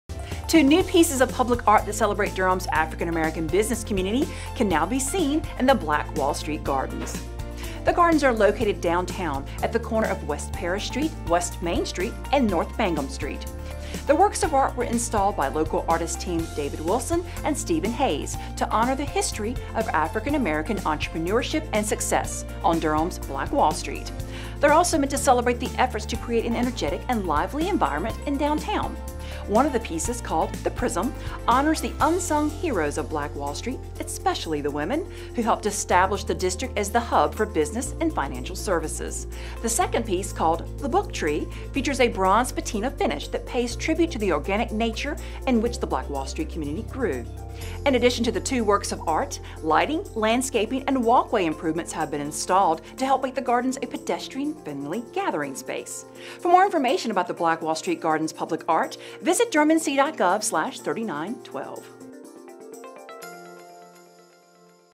Bull City Today ep.1029 Black Wall Street Gardens [June 20 2022]
Two new pieces of public art that celebrate Durham’s African American business community can now be seen in the Black Wall Street Gardens. Get a glimpse of how they’re helping to honor the history of African American entrepreneurship and success on BullCityToday.

Transcript: Two new pieces of public art that celebrate Durham’s African American business community can now be seen in the Black Wall Street Gardens. The gardens are located downtown at the corner of West Parrish Street, West Main Street, and North Mangum Street. The works of art were installed by local artist team David Wilson and Stephen Hayes to honor the history of African American entrepreneurship and success on Durham’s Black Wall Street. They’re also meant to celebrate the efforts to create an energetic and lively environment in downtown. One of the pieces, called The Prism, honors the unsung heroes of Black Wall Street, especially the women, who helped establish the district as the hub for business and financial services. The second piece, called The Book Tree, features a bronze patina finish that pays tribute to the organic nature in which the Black Wall Street community grew. In addition to the two works of art, lighting, landscaping, and walkway improvements have been installed to help make the gardens a pedestrian-friendly gathering space.
For more information about the Black Wall Street Gardens public art, visit Durham NC dot gov slash 3912.